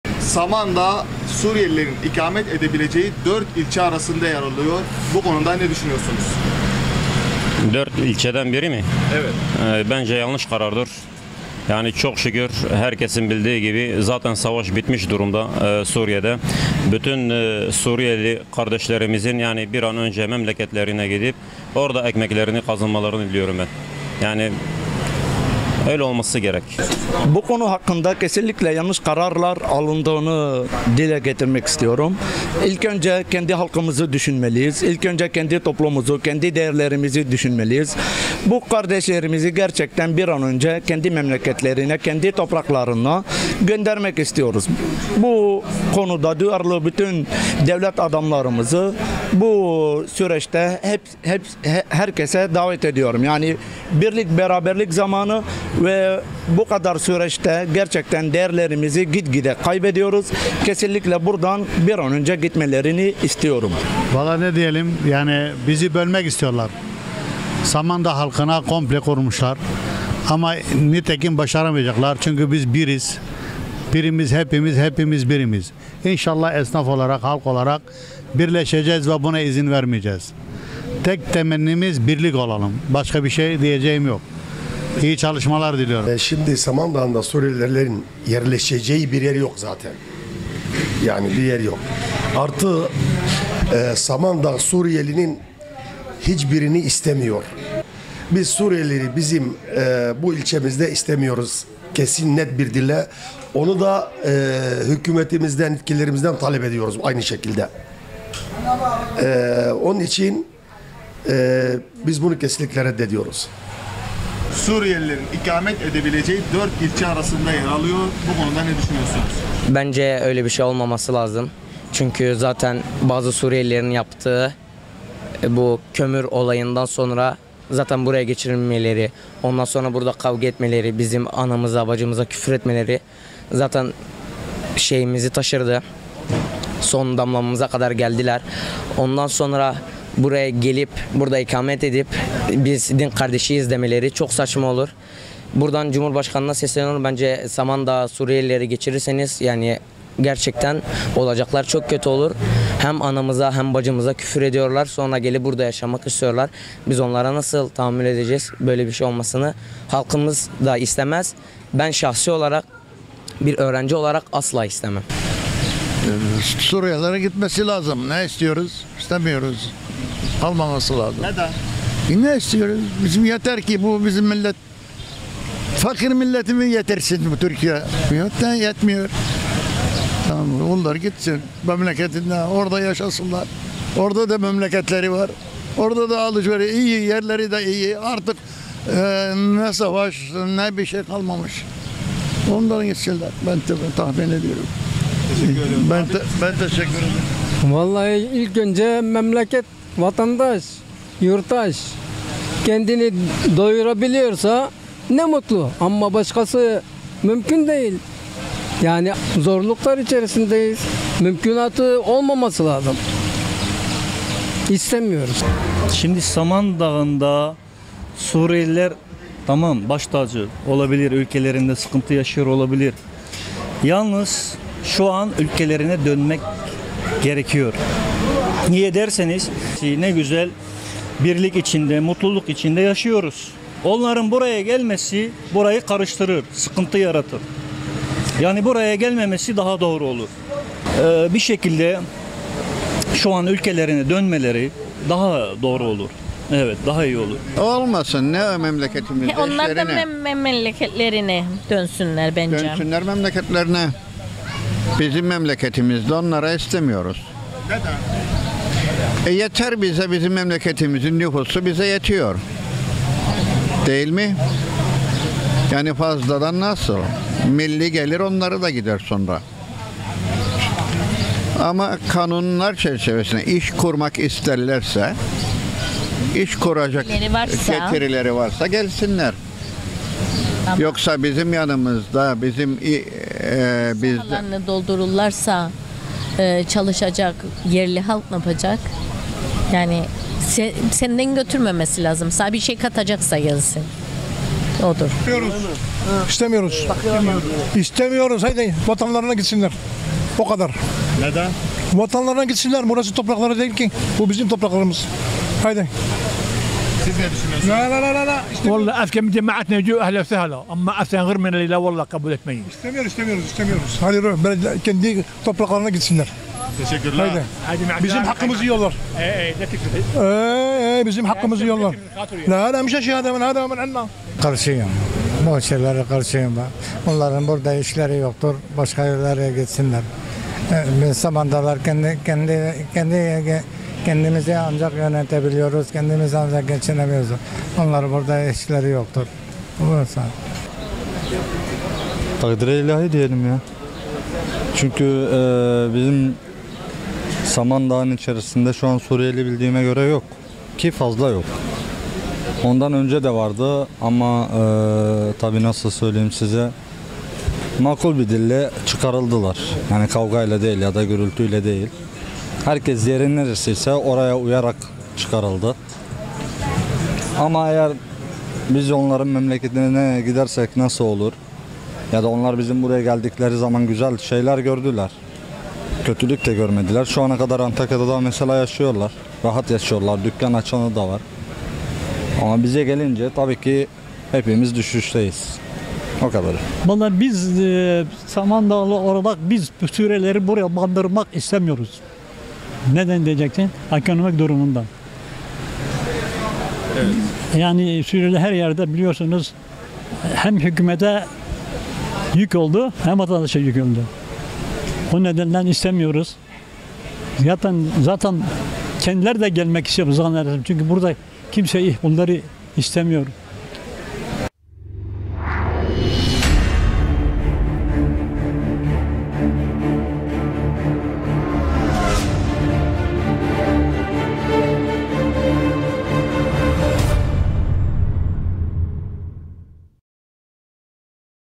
SAMANDAĞ'da SURİYELİLER KARARINA NET CEVAPLAR!
Samandağ'da Sokak Röportajımız
Samandağ'ın Suriyelilerin ikamet edebileceği 4 ilçeden biri olması hakkında ne düşünüyorsunuz?